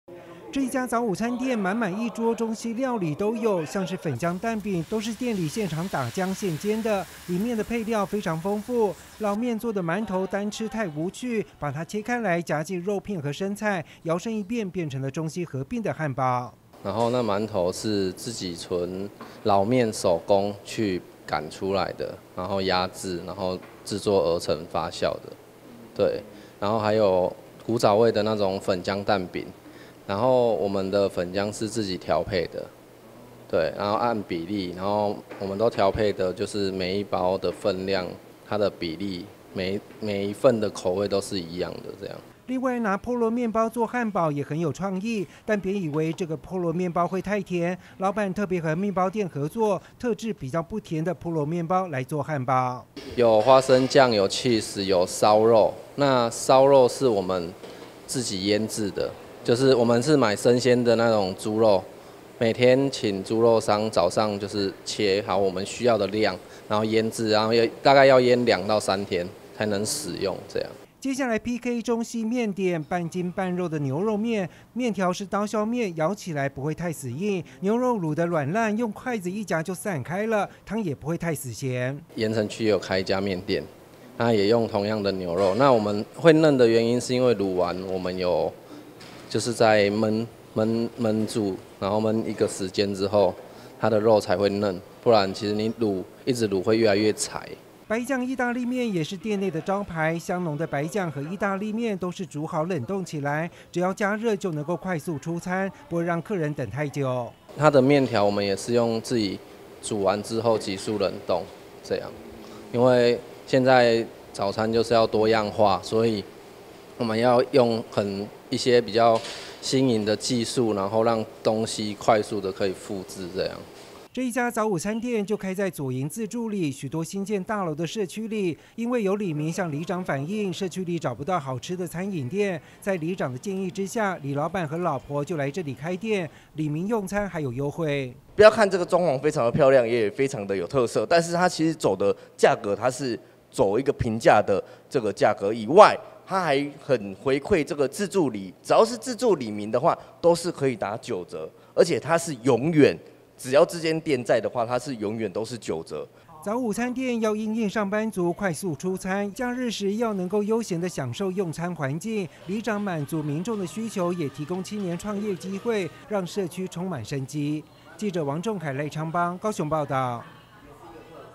樓下美食 | 早午餐店也有好料理 中西合併創意美食加分
左營區自助里有許多住宅大樓，缺少餐飲店家進駐；在里長的鼓勵之下，一對年輕夫妻就在裡面開了一間早午餐店；除了牛肉麵的牛肉滷得很軟爛之外，白醬義大利麵，也非常香濃；更有饅頭和波蘿麵包做的創意漢堡，中西合併，很有特色。